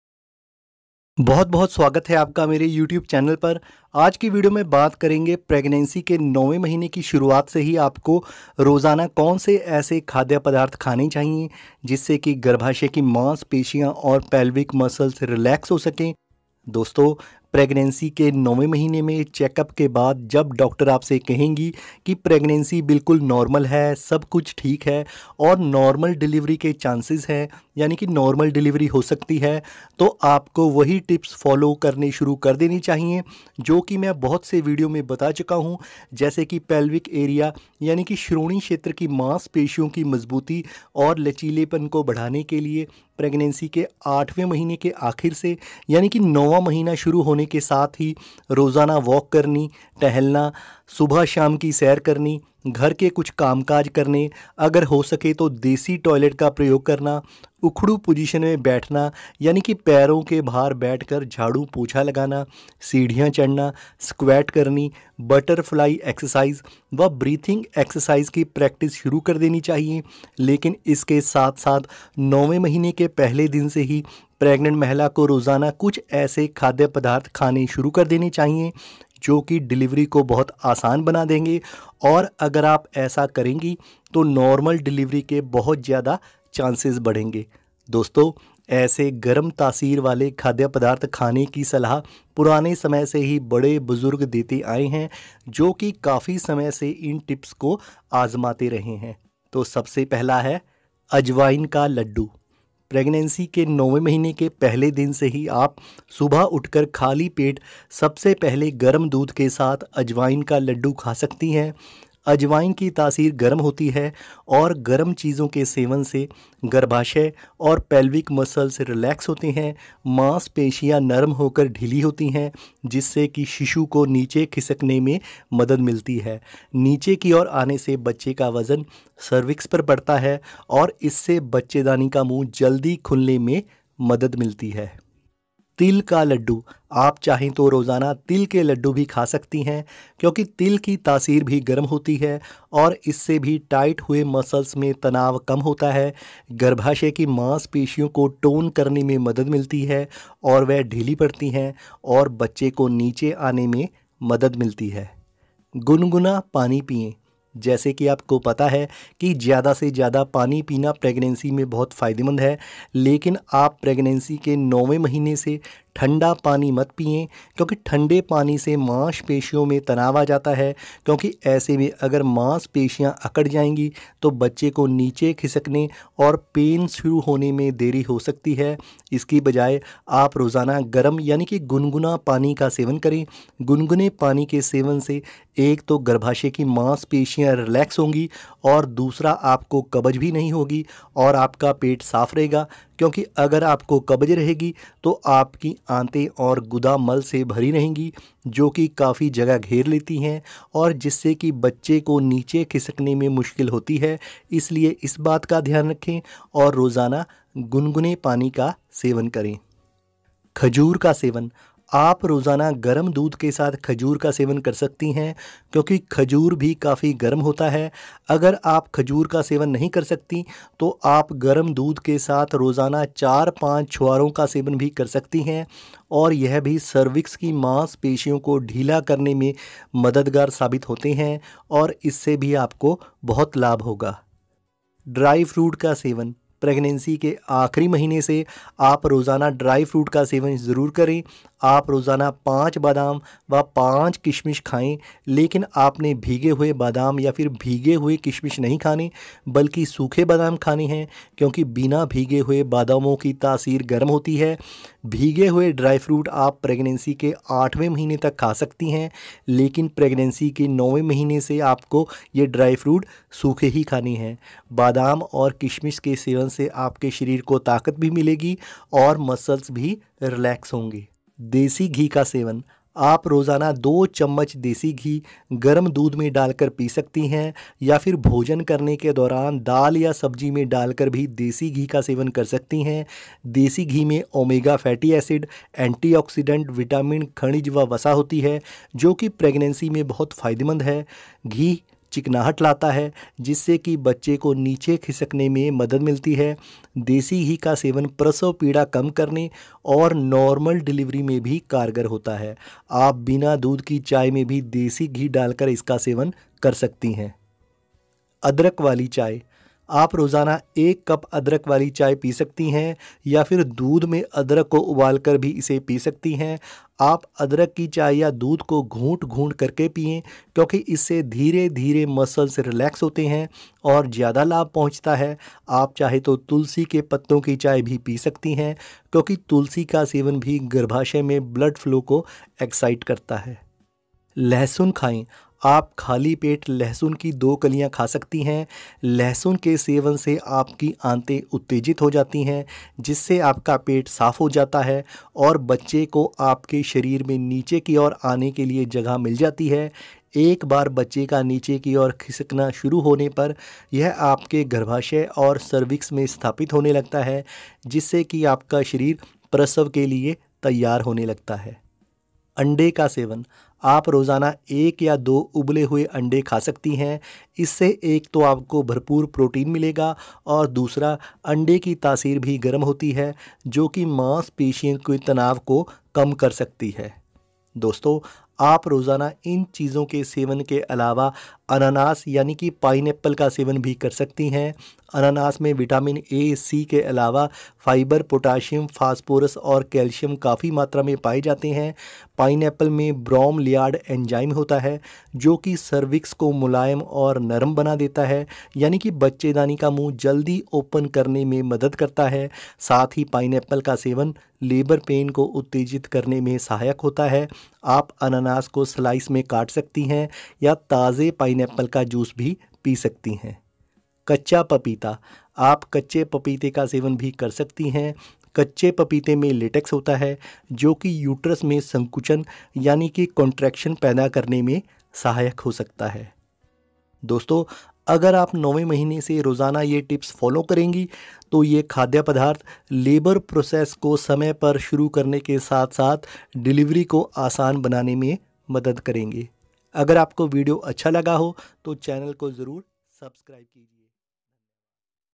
9वें महीने ये खाएं, होंगे बहुत फायदे
9वें महीने में ये खाएं

इस वीडियो में बताऊंगा कि गर्भावस्था के नौवें महीने की शुरुआत से ही आपको रोजाना कौन से ऐसे खाद्य पदार्थ खाने चाहिएं, जिससे गर्भाशय की मांसपेशियां और पेल्विक मसल्स रिलैक्स हो सकें, बच्चा जल्दी नीचे की ओर खिसक सके, समय पर खुद-ब-खुद लेबर पेन शुरू हो जाए, सरविक्स यानी बच्चेदानी का मुंह अपने आप खुलना शुरू हो जाए और डिलीवरी में कम समय लगे, तो इसके लिए वीडियो आखिर तक देखें और अगर पसंद आए तो लाइक जरूर कीजिए।

डिलीवरी से संबंधित महत्वपूर्ण वीडियोज़ :-